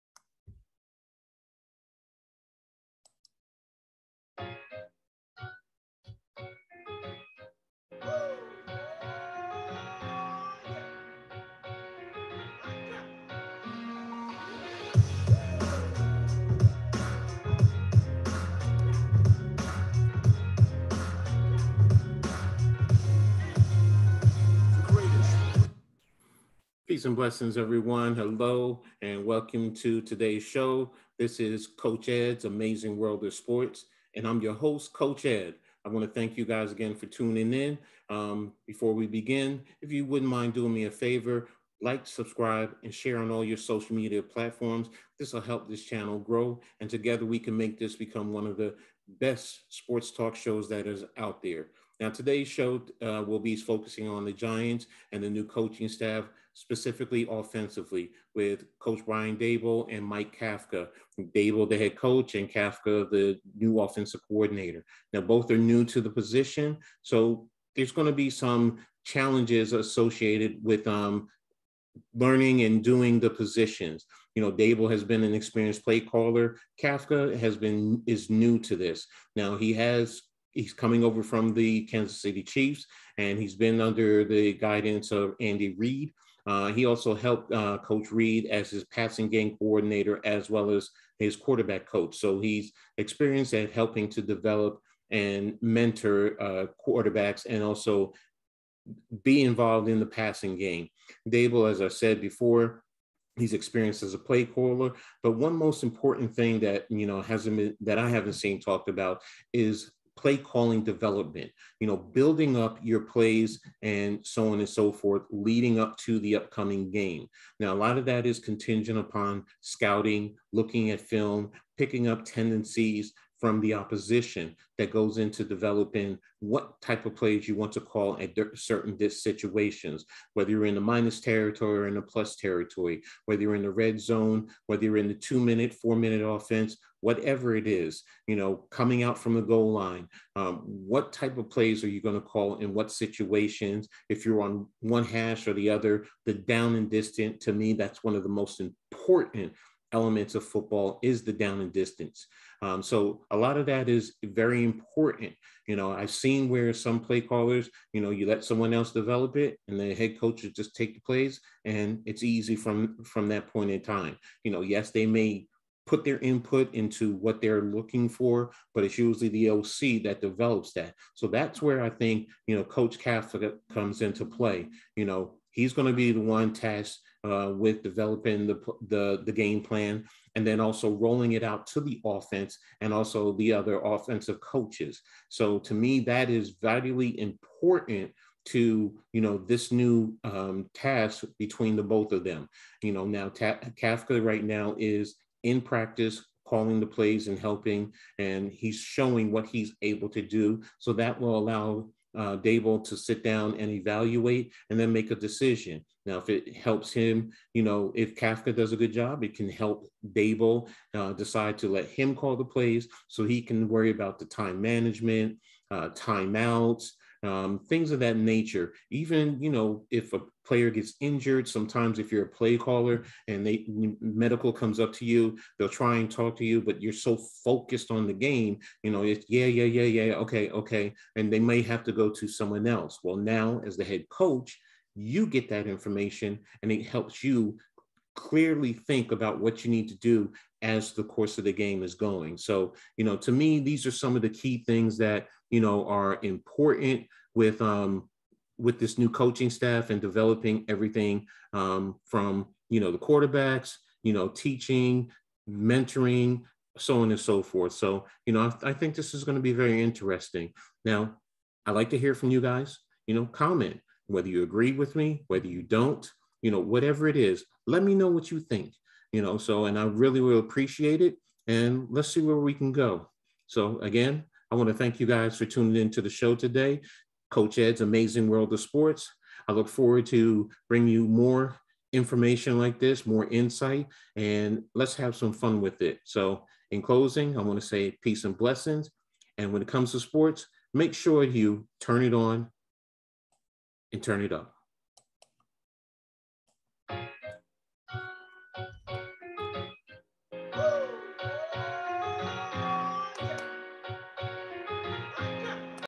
Coach Edd's Amazing World of Sports/play calling NY Giants
How does play calling impact the new offense? Can Kafka ascend to calling the offensive play?

Offensive development is as important to play calling for the NY Giants.